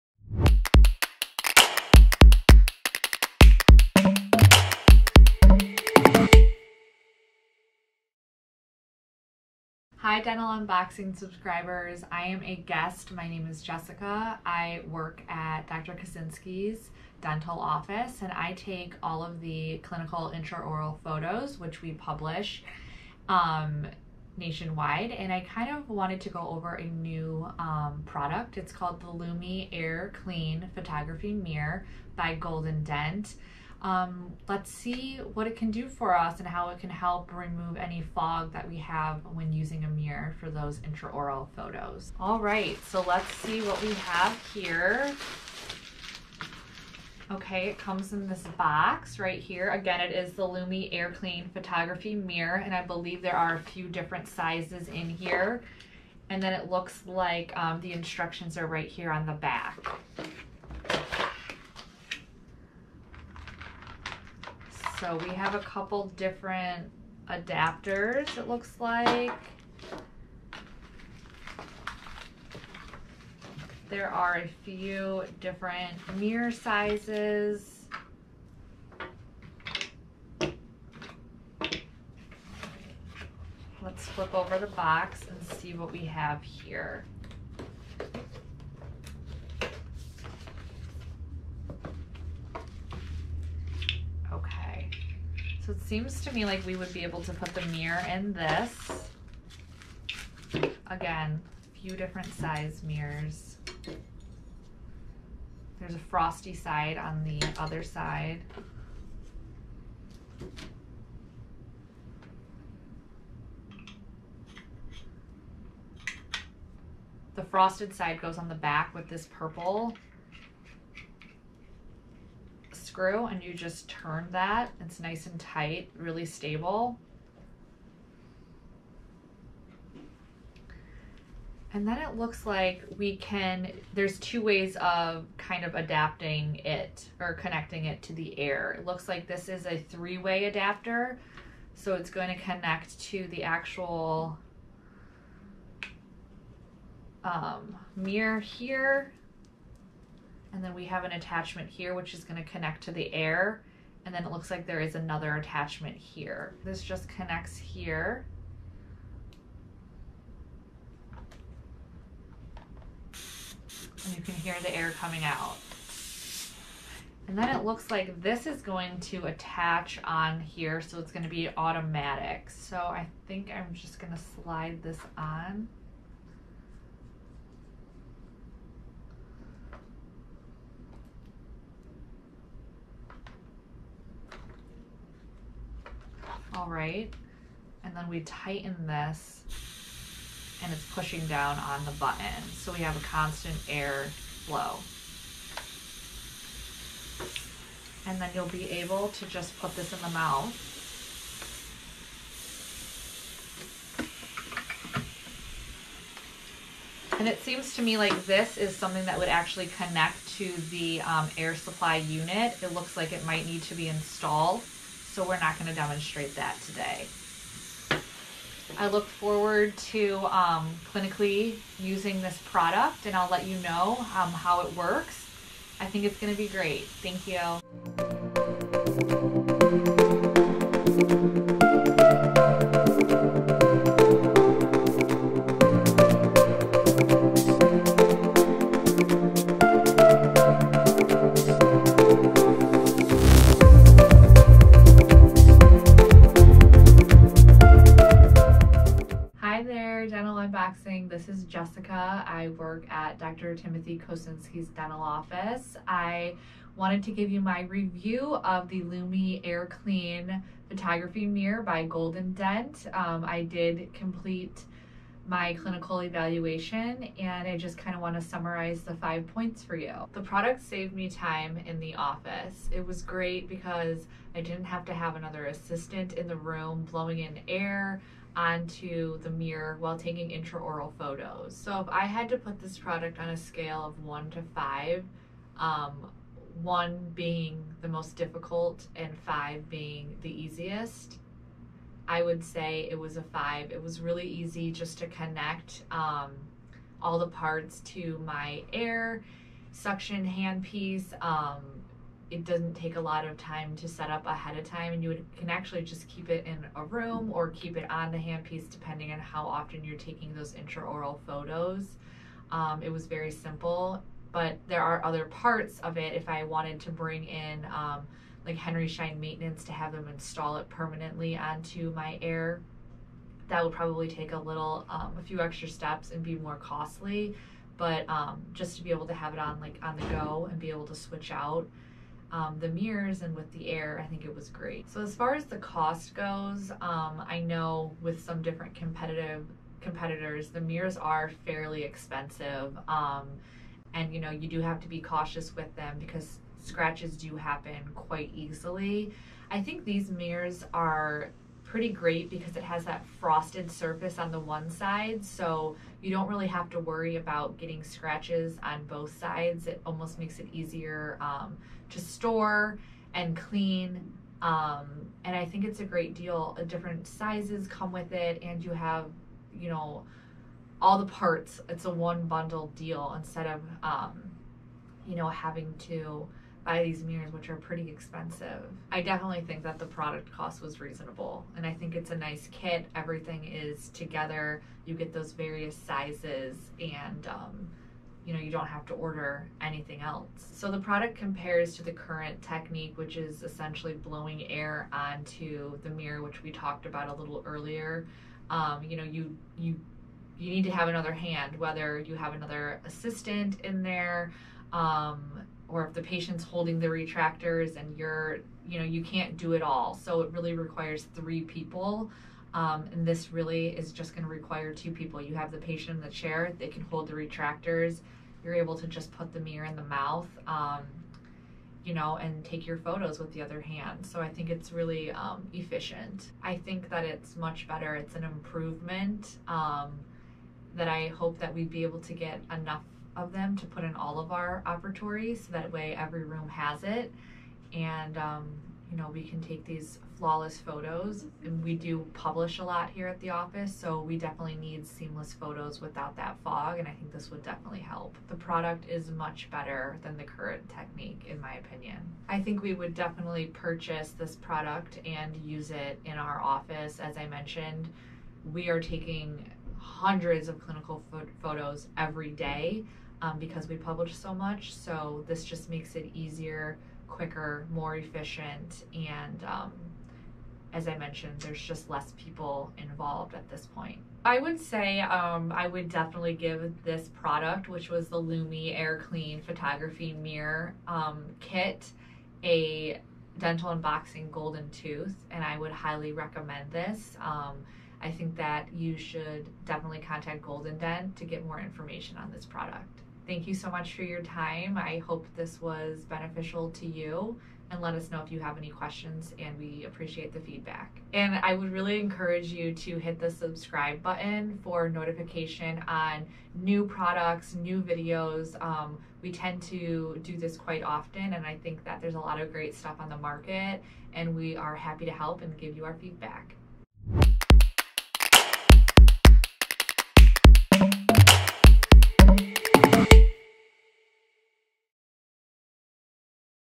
Dental Unboxing & Evaluation of Lumi AirClean Photography Mirrors, Dental Photography Mirrors
In this video we complete a Dental Unboxing of the Lumi AirClean Dental Photography Mirrors and clinical evaluation compared to other conventional photo mirrors that do not have an air connection.

Product Includes:

Buccal Double Sided Mirror: Qty 1
Lingual Double Sided Mirror: Qty 1
Occlusal Small: Qty 1
Occlusal Medium: Qty 1
Occlusal Large: Qty 1
Mirror Handles: Qty 2
Air Connections & Tubing Kit: Qty 1

Product Highlights

* Self Clean Air Photography Mirrors Use Air to Clean Lens
* Unparalleled Working Photography Visibility Without Distraction
* Easy & Instant Plug & Use Air Connect Options - Easy Set Up
* Autoclaveable Medical-Grade for Optimal Hygiene

Product Description
This patented Self Clean Air Dental Photography Mirror system combines an Ultra High Definition front surface alloy coated mirrors with an industry best reflectance rate of 98% with quick connect air tubing for unparalleled working photography visibility without distraction. The system has two instant plug and use connect options to easily connect the air mirror to your air/water syringe or your dental unit with ease in a few minutes or less without hassle. The quick connect tubing is light-weight and allows for 360 degree free rotation of the handles with minimal drag.

The Dental Mirrors are autoclaveable up to 500 cycles at 135 C.

Easily disconnect the mirrors handles from the air tubing in seconds with the quick connect design. Mirrors can also be used without air flow if this is preferred. See part # M5NA if you prefer mirrors without air clean option.

The photography mirrors come in the following types:

Buccal Double Sided Mirror
Lingual Double Sided Mirror
Occlusal Small
Occlusal Medium
Occlusal Large